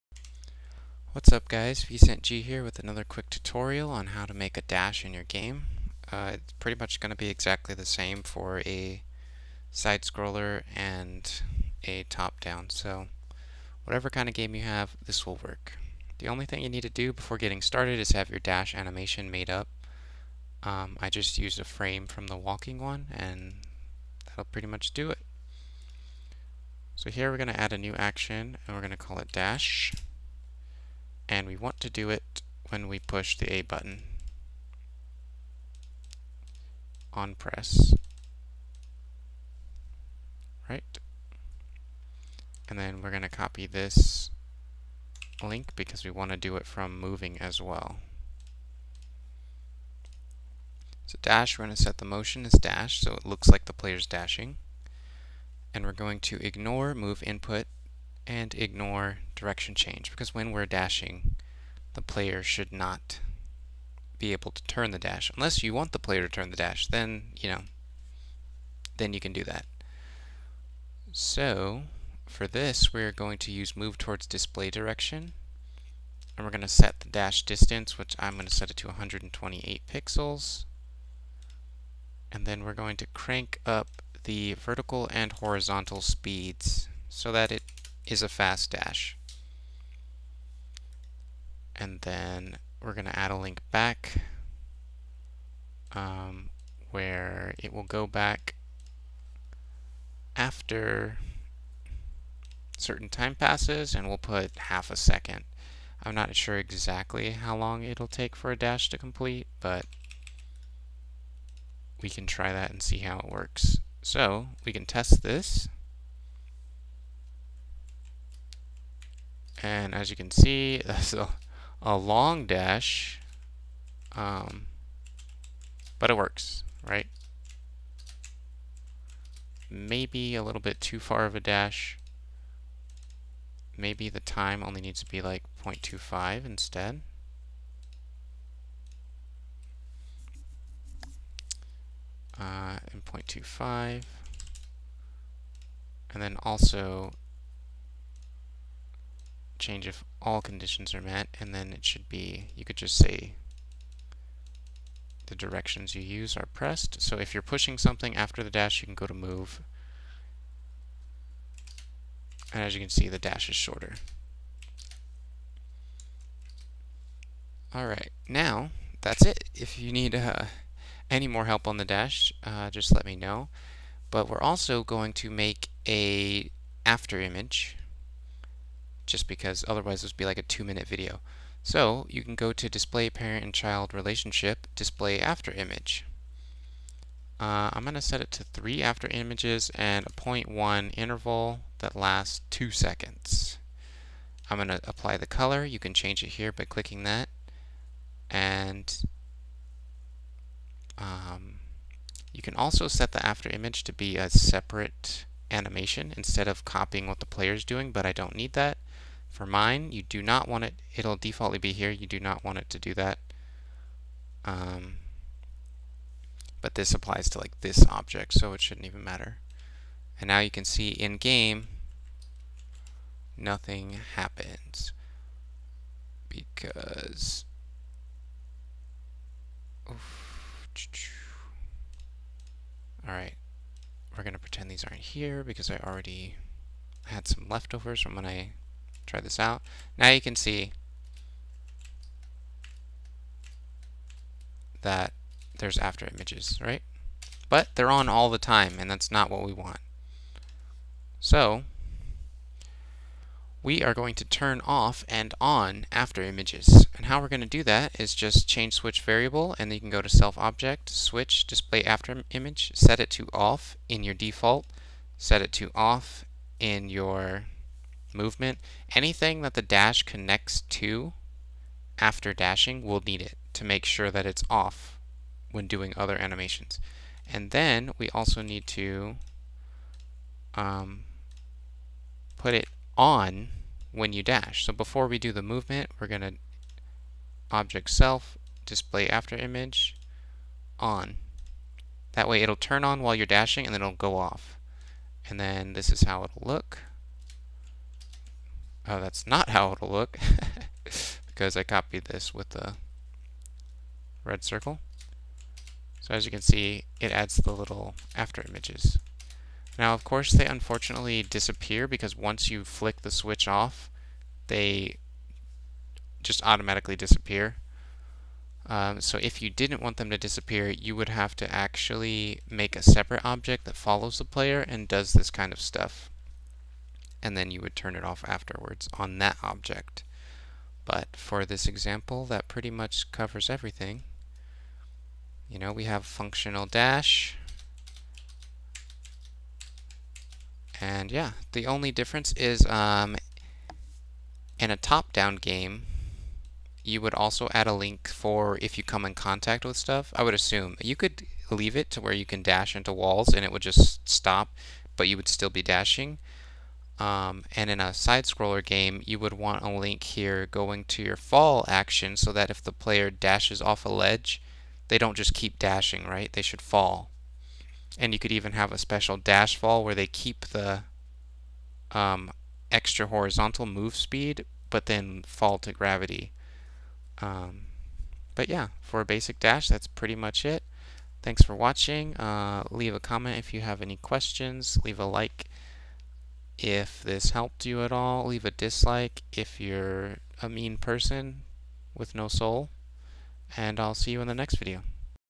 Dash Tutorial - Pixel Game Maker MV
Probably the quickest and simplest way to make a dash in your game.